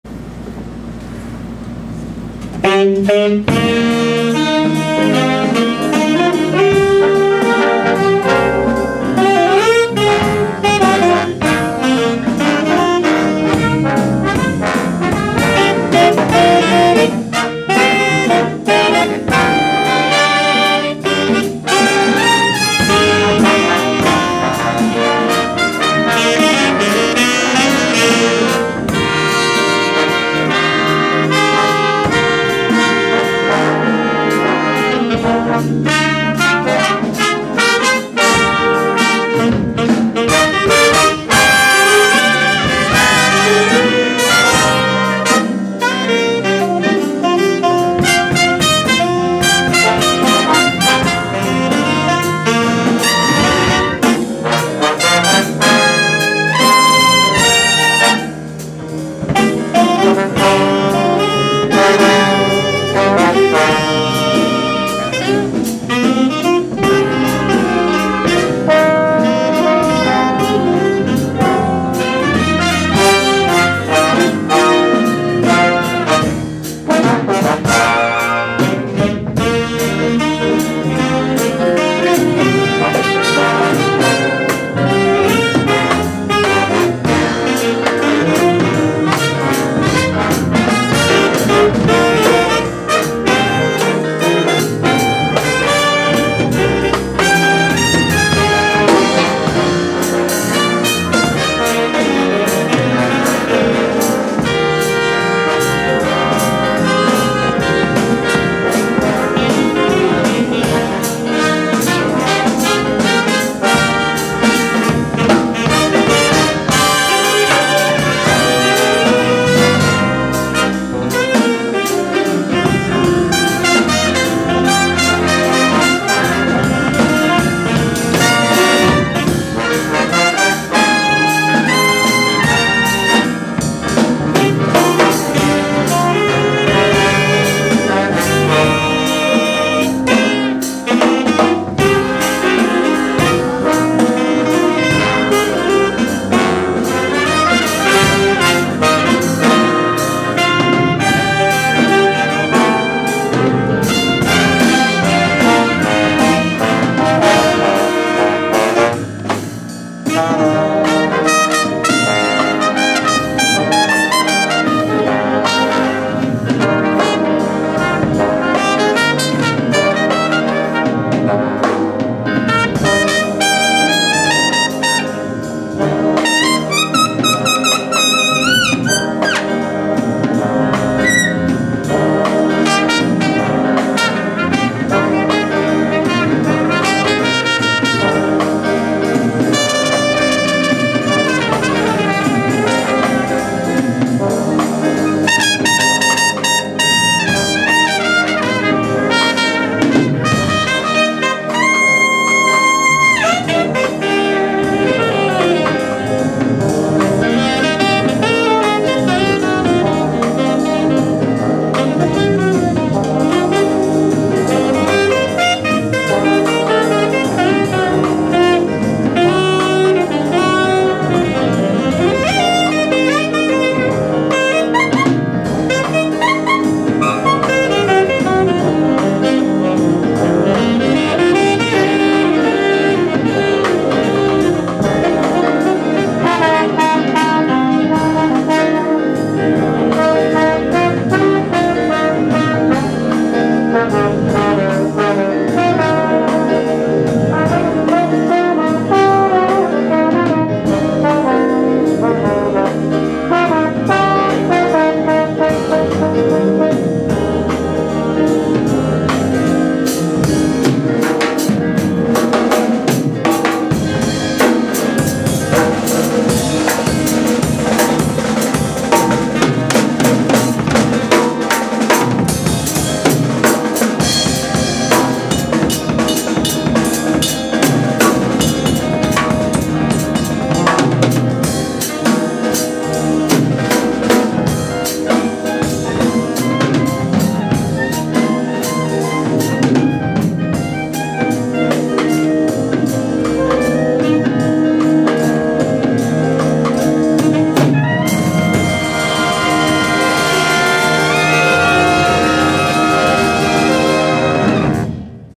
banda da brigada de tefé
para despedir dos companheiros transferidos e mostrar que no meio da selva amazônica também se faz música de qualidade!